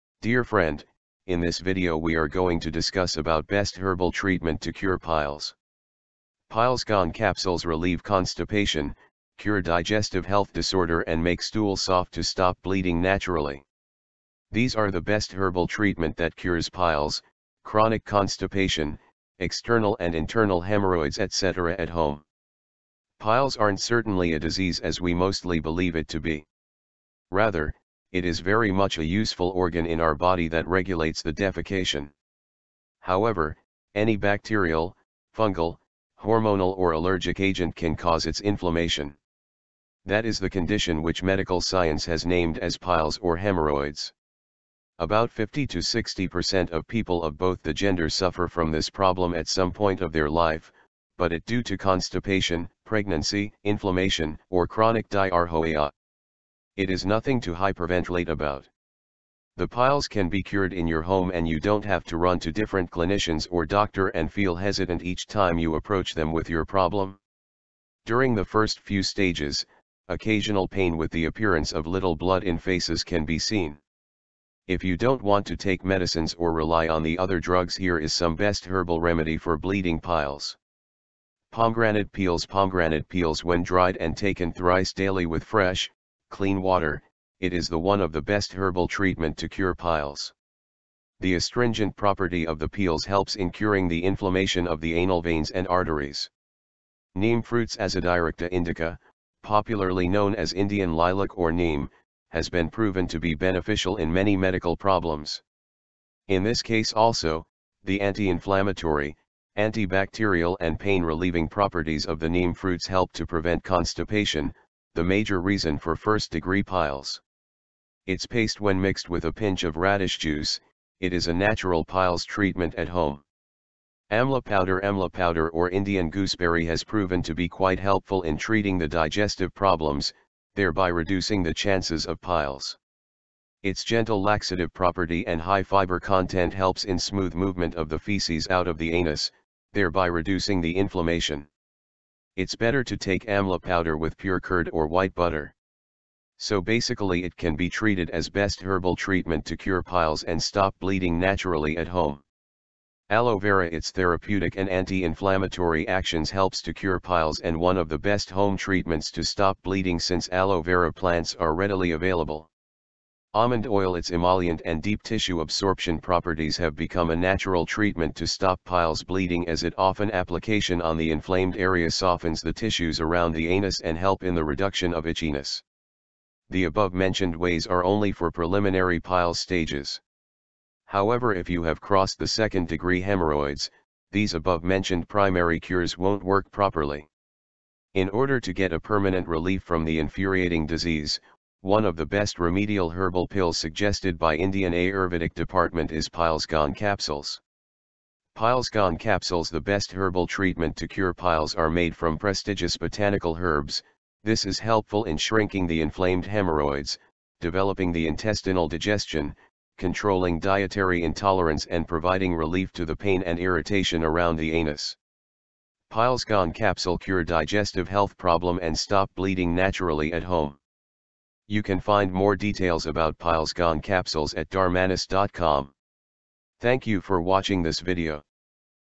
Best Herbal Treatment to Cure Piles, Stop Bleeding Naturally at Home
Dear friend, in this video we are going to discuss about best herbal treatment to cure piles. Pilesgon capsules relieve constipation, cure digestive health disorder and make stool soft to stop bleeding naturally. These are the best herbal treatment that cures piles, chronic constipation, external and internal hemorrhoids etc. at home.